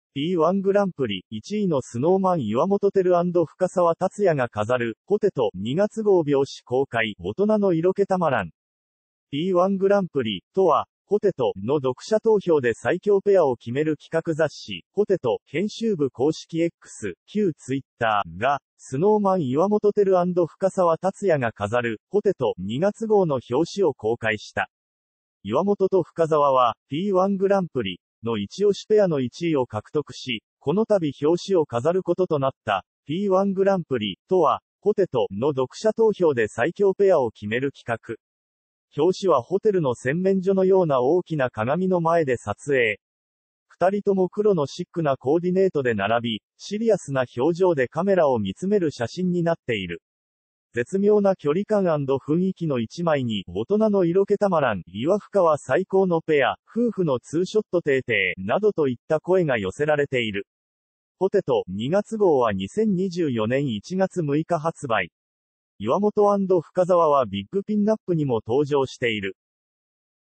P-1グランプリ!!1位のSnow Man岩本照＆深澤辰哉が飾る『POTATO』2月号表紙公開「大人の色気たまらん」
■「P-1グランプリ!!」とは、『POTATO』の読者投票で最強ペアを決める企画 雑誌『POTATO』編集部公式X（旧Twitter）が、Snow Man岩本照＆深澤辰哉が飾る『POTATO』2月号の表紙を公開した。
岩本と深澤は「P-1グランプリ!!」のイチオシペアの1位を獲得し、このたび表紙を飾ることとなった。「P-1グランプリ!!」とは、『POTATO』の読者投票で最強ペアを決める企画。 表紙はホテルの洗面所のような大きな鏡の前で撮影。ふたりとも黒のシックなコーディネートで並び、シリアスな表情でカメラを見つめる写真になっている。 絶妙な距離感＆雰囲気の一枚に「大人の色気たまらん」「いわふかは最高のペア!!」「夫婦のツーショットてえてえ」などといった声が寄せられている。 『POTATO』2月号は2024年1月6日発売。岩本＆深澤はBIGピンナップにも登場している。

IMPORTANT!